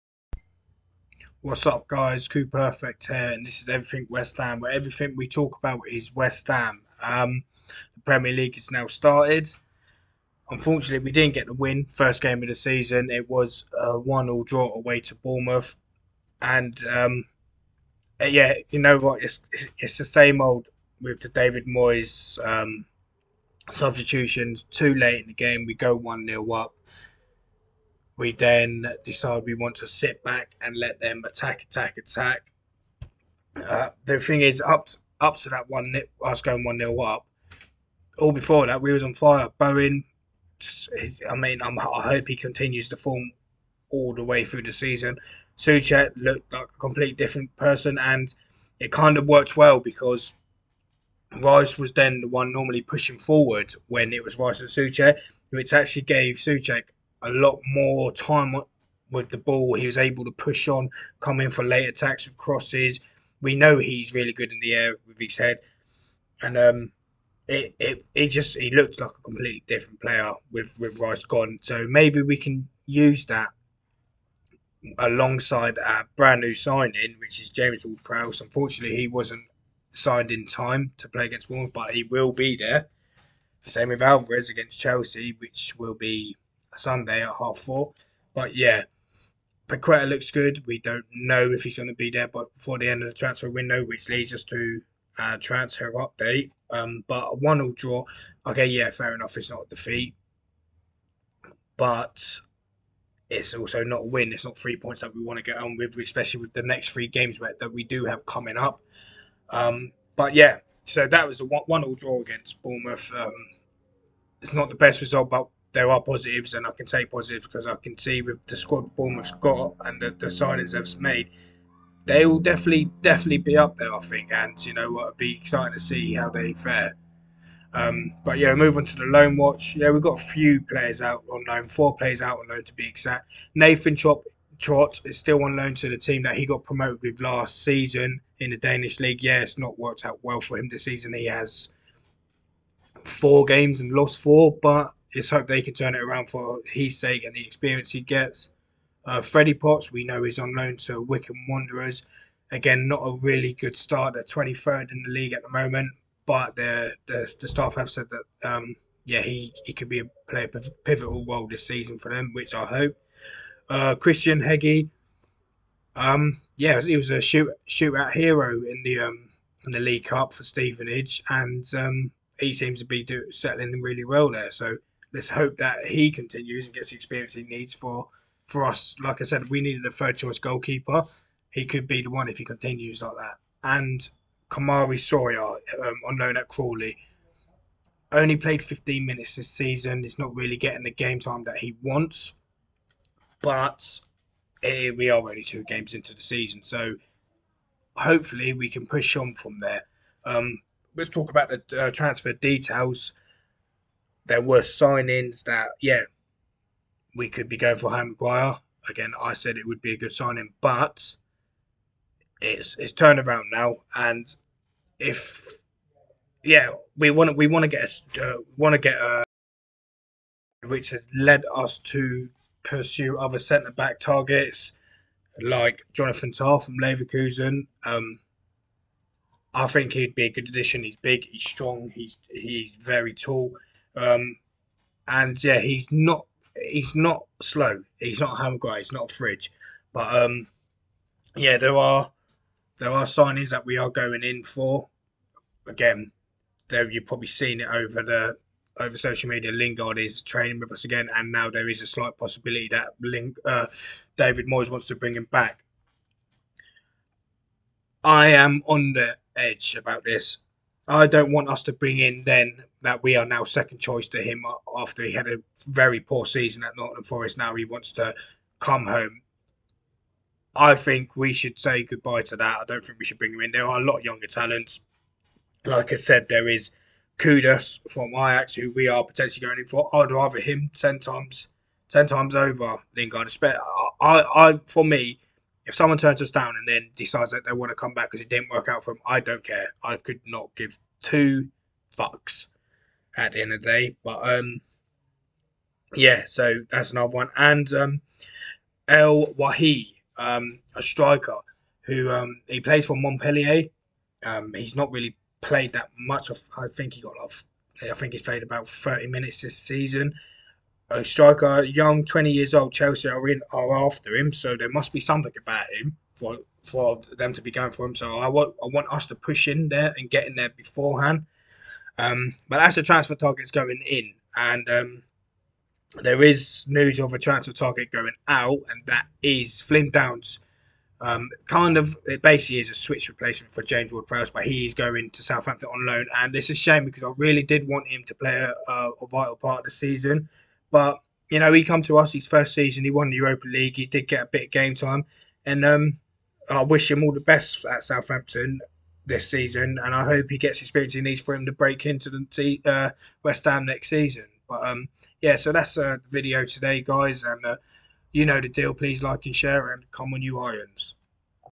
Everything West Ham - Bournemouth result loan report and transfer news 16/08/23
talking bournemouth result, our loan reports and tranfer news